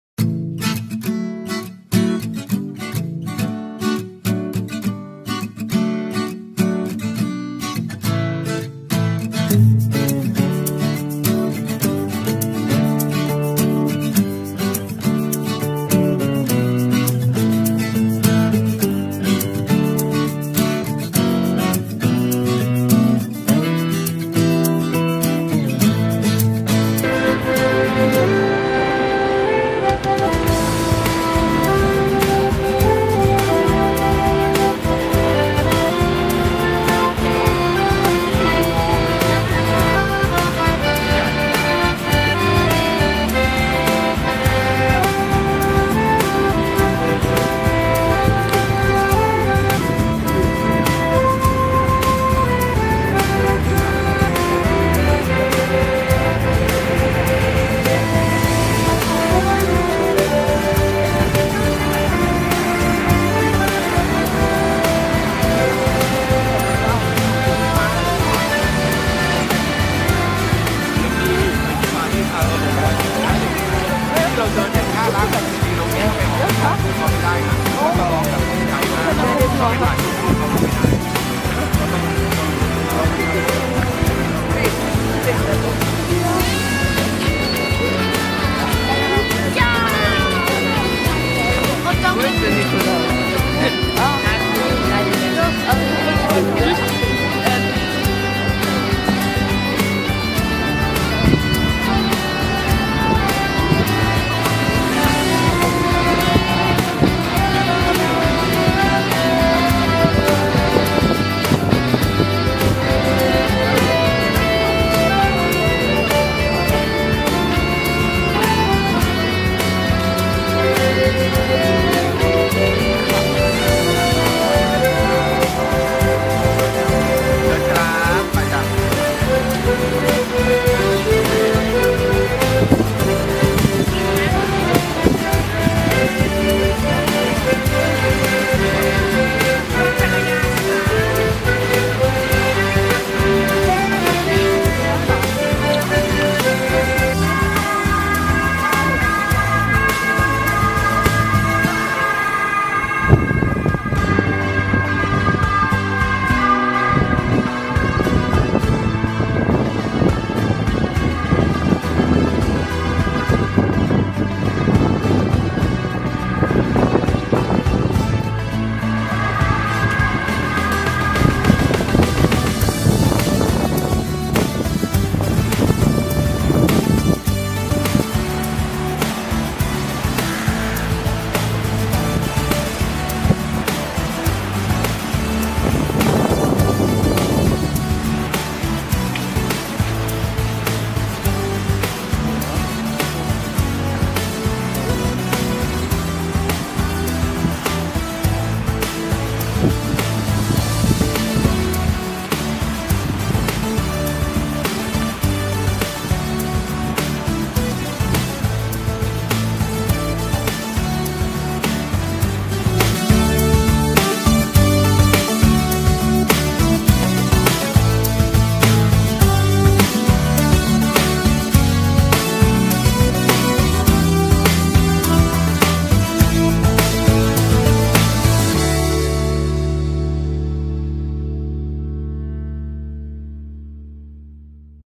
Hallstatt หมู่บ้านมรดกโลก..... ประเทศออสเตรีย
ภาพ   :   บรรยากาศท่องเที่ยว  Hallstatt   หมู่บ้านมรดกโลก  ในออสเตรีย
เพลง  :   เพลงบรรเลงเข้าบรรยากาศ